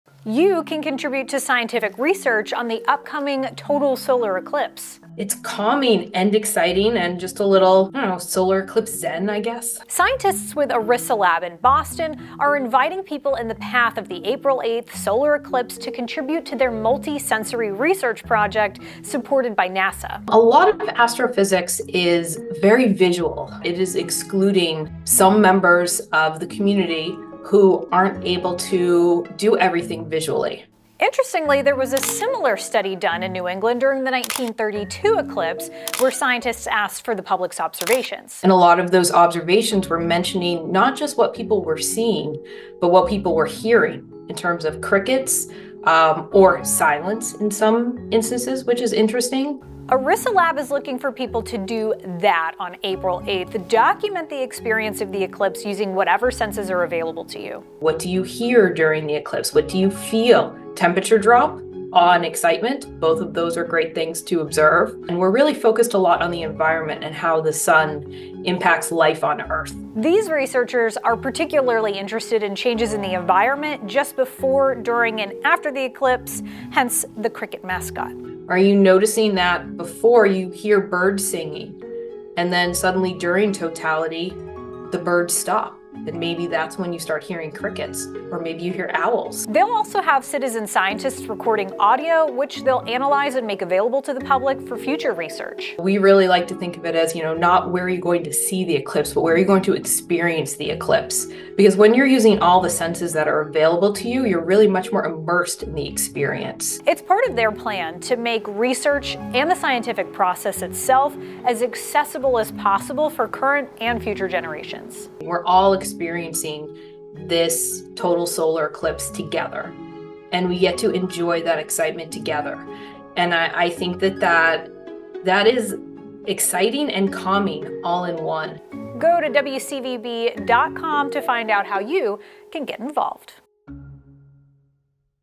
Your role during the eclipse
A Boston-based lab invites people in the path of the April 8 solar eclipse to contribute to a multisensory research project.